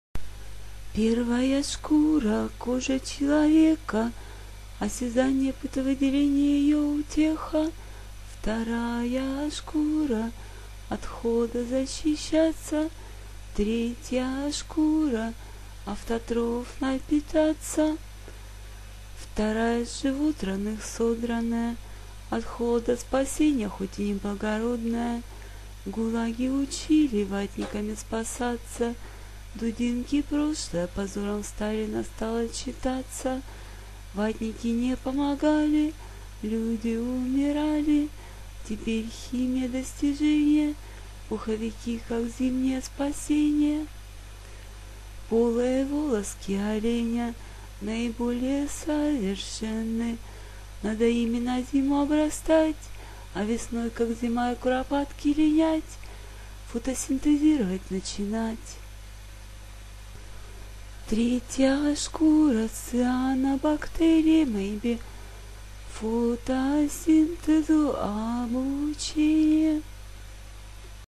три шкуры человека
Первая шкура - кожа человека
Осязание, потовыделение её утеха.
Вторая шкура - от холода защищаться,
Третья шкура - автотрофно питаться.

Вторая - с животных содранная,
От холода спасение, хоть и не благородное.
В Гулаге учили ватниками спасаться,
Дудинки прошлое - позором Сталина стало считаться,
Ватники не помогали,
Люди умирали.
Теперь - химии достижение,
"Пуховики" как зимнее спасение.

Полые волоски оленя
Наиболее совершенны,
Надо ими на зиму обрастать,
А весной как заяц и куропатка линять,
Фотосинтезировать начинать!

Третья - с цианобактерией?
Фотосинтезу обучение.